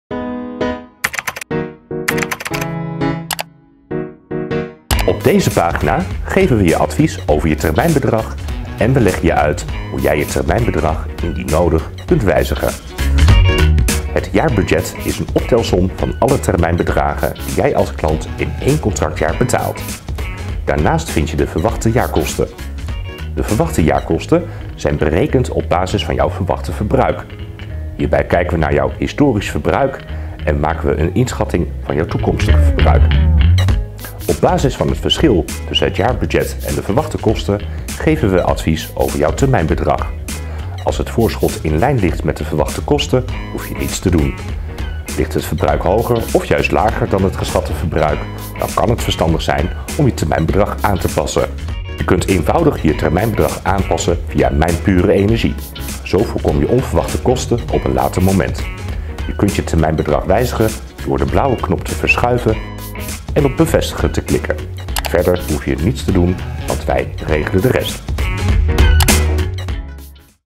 Je termijnbedrag aanpassen via Mijn Pure Energie
Door regelmatig je energieverbruik te bekijken, weet je zeker of jouw vaste termijnbedrag overeenkomt met jouw verbruik. Soms is het verstandig om je termijnbedrag aan te passen om latere kosten te voorkomen. Je ontvangt dan van ons een melding. Je termijnbedrag kun met één klik eenvoudig aanpassen via Mijn Pure Energie.

Inloggen kan met je klantnummer of e-mailadres dat bij ons bekend is en jouw persoonlijke wachtwoord. De inloggegevens voor je persoonlijke account krijg je na je aanmelding via e-mail toegestuurd.